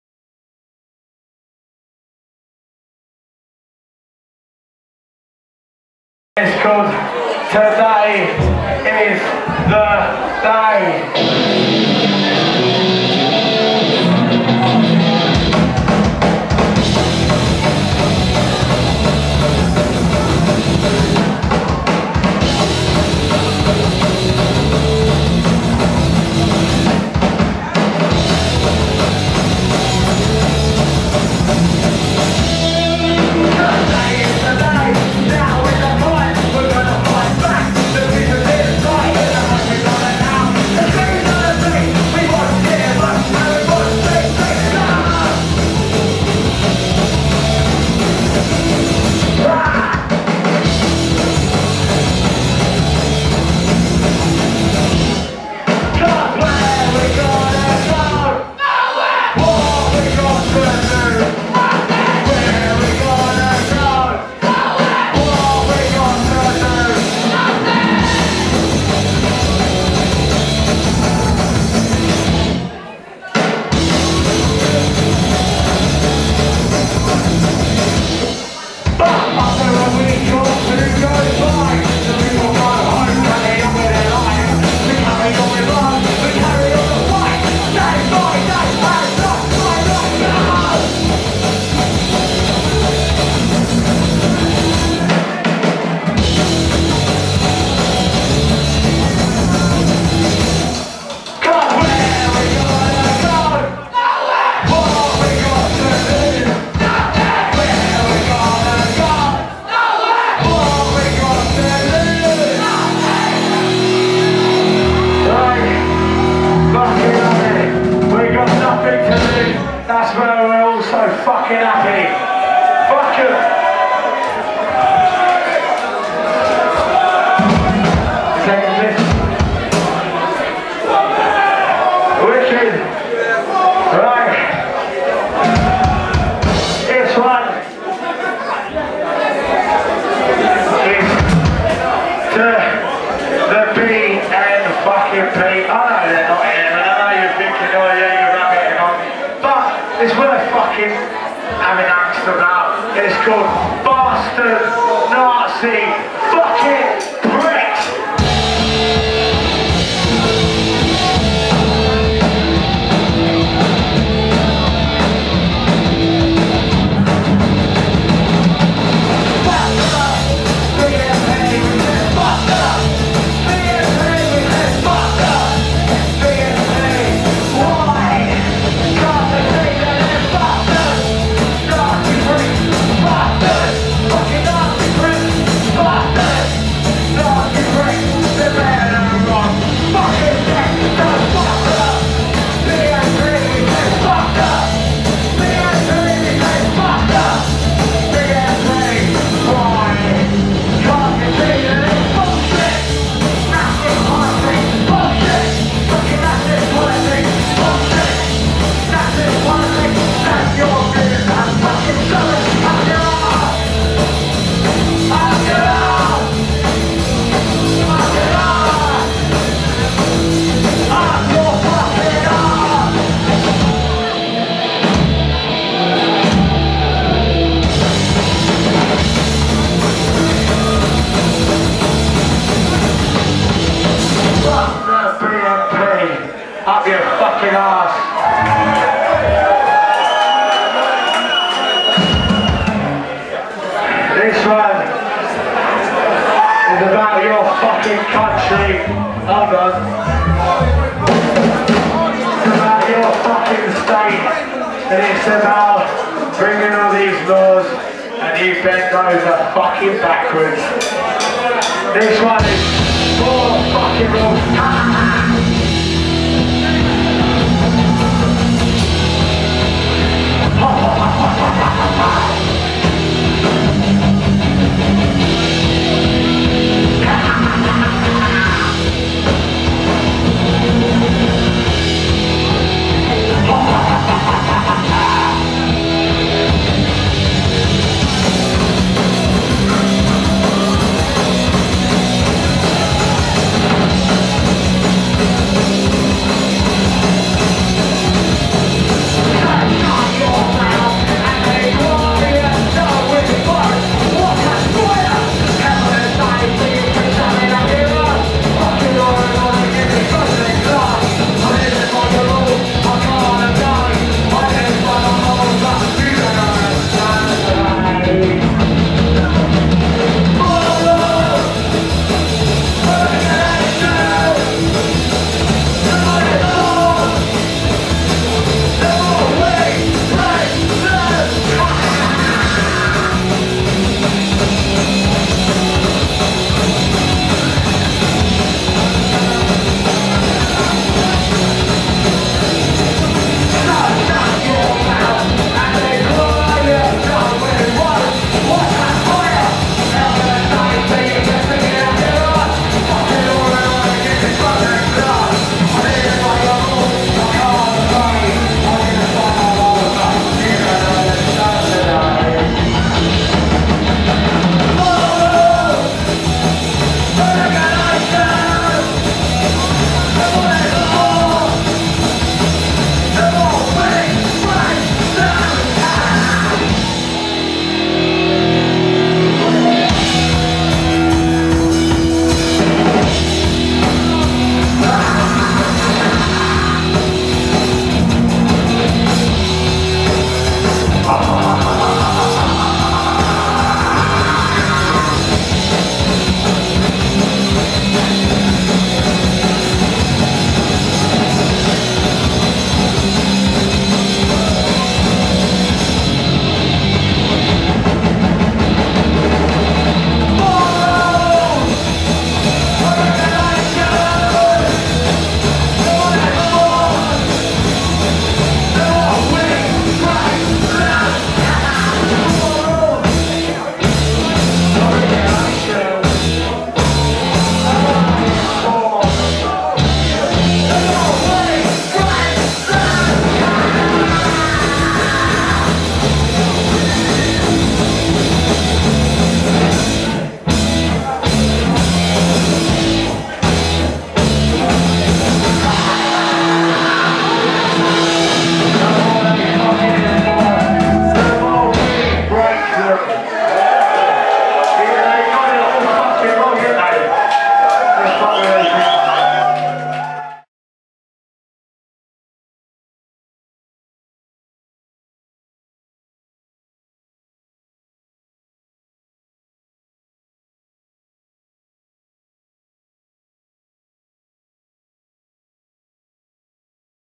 Nuke on Route part 1 Gary Critchley benefit 2 Oct 2010
Nuke on Route filmed at the Justice for Gary Critchley benefit at the Bussey Building, Peckham, 2 October 2010 part 1 of 2, so more to come! There was a very over-zealous smoke machine in action, hence the band looking rather shadowy and grainy in this particular clip, although the sound is good.  Find out more about Gary Critchley, and why his case is one of the most serious miscarriages of British justice,
this event was also part of a 10-day mass extravaganza of socially, politically & musically-charged world class creativity, hosted by thearmed909...E.R.my arse...